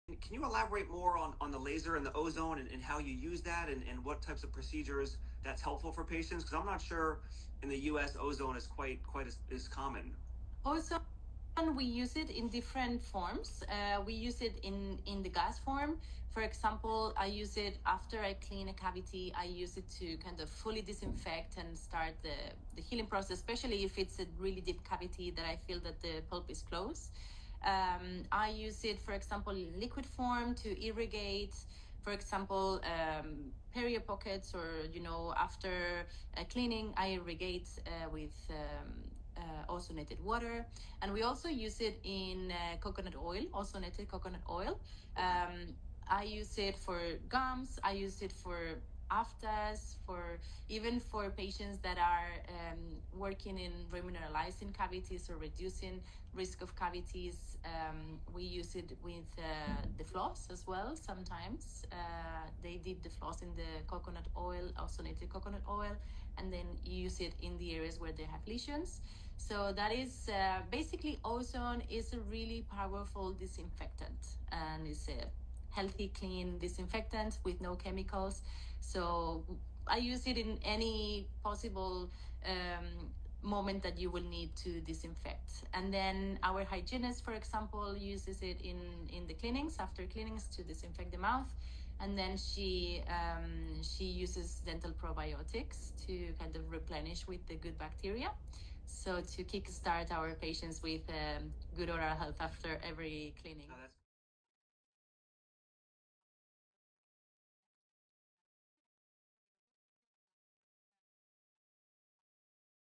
What’s the role of Ozone Therapy in dental care
Unlocking the power of Ozone Therapy with Dr. Derek Gatta and Dr. Laura Maanon! 🌿✨ From combating bacteria to promoting faster healing, this innovative approach is revolutionising dental care.